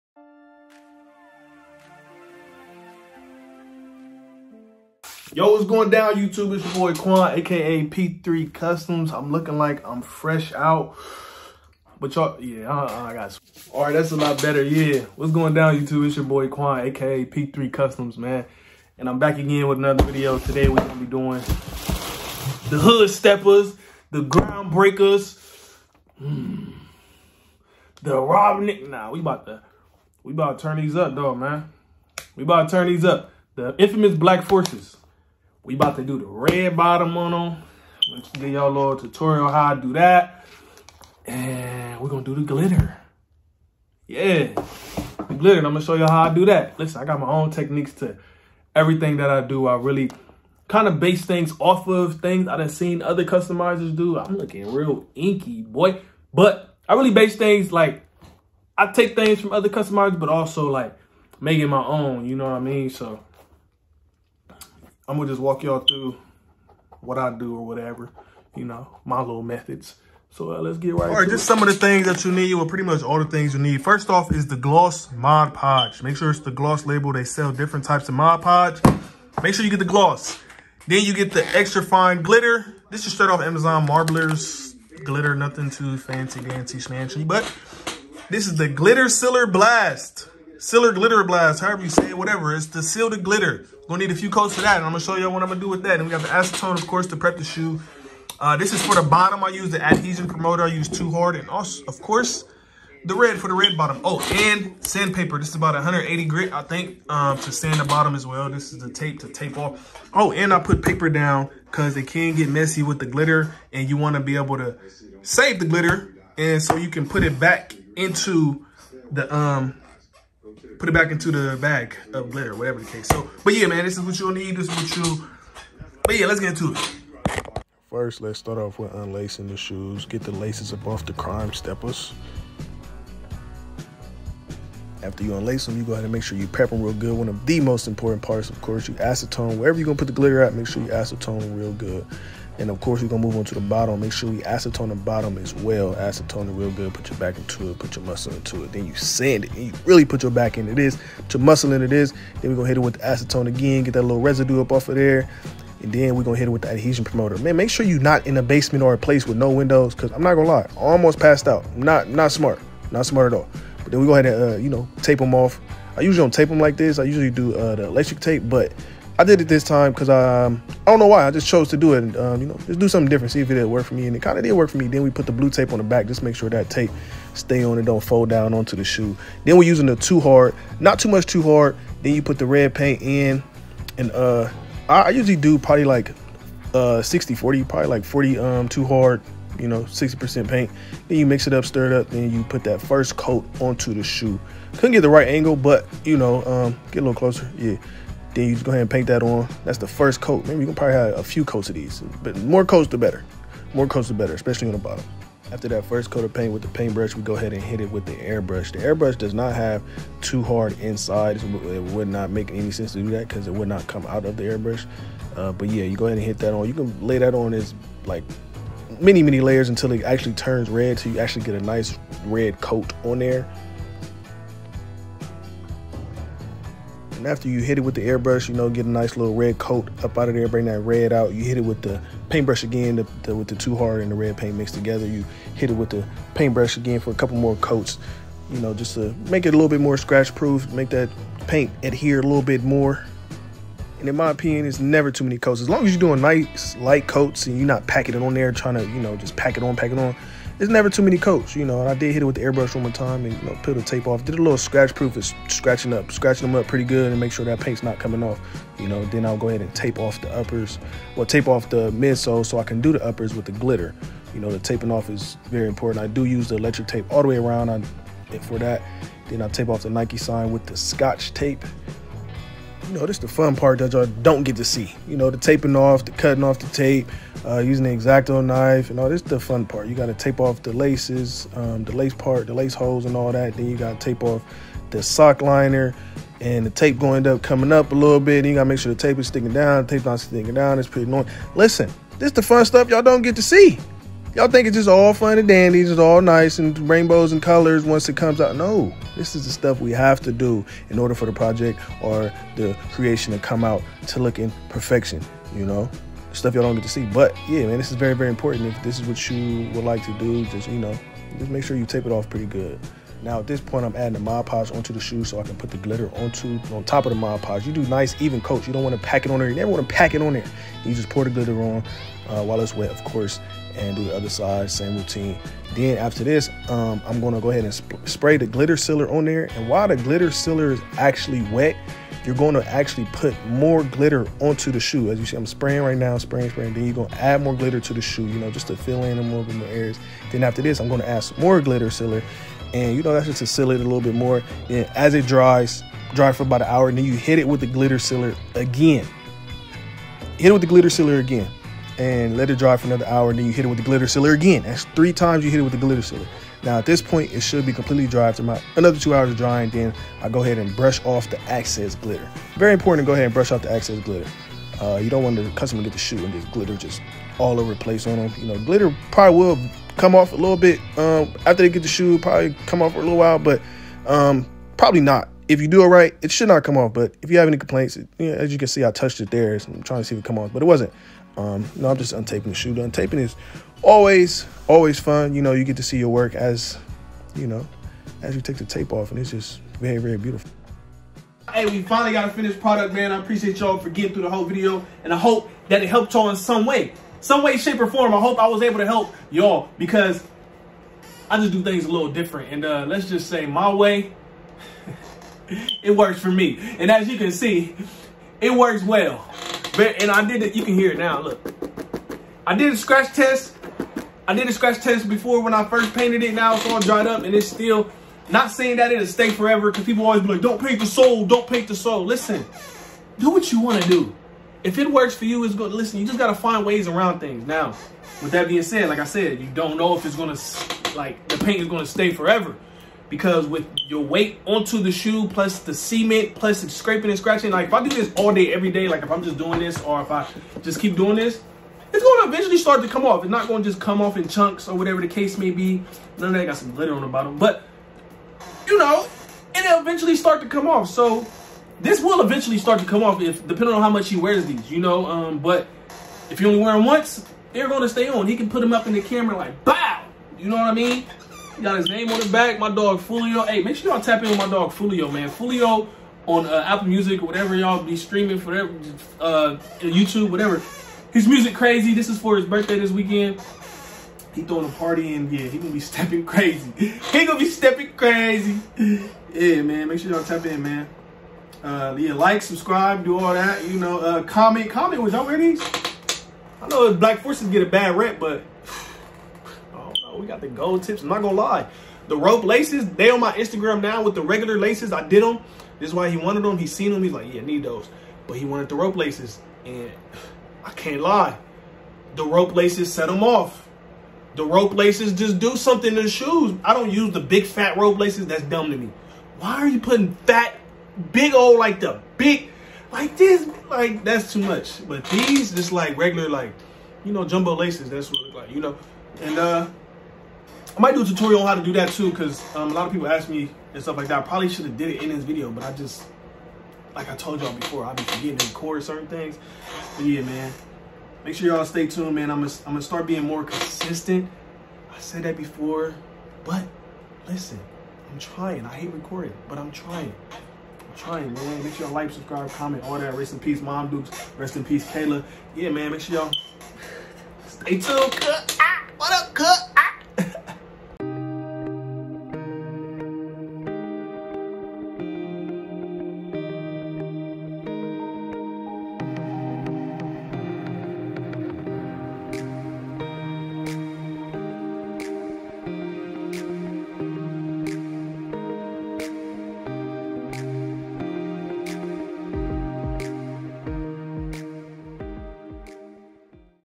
TUTORIAL: Black Glitter red bottom custom AF1
Mostly All products in the video were purchased through Amazon. Let me know if the video was helpful and what's the next tutorial you'd like to see.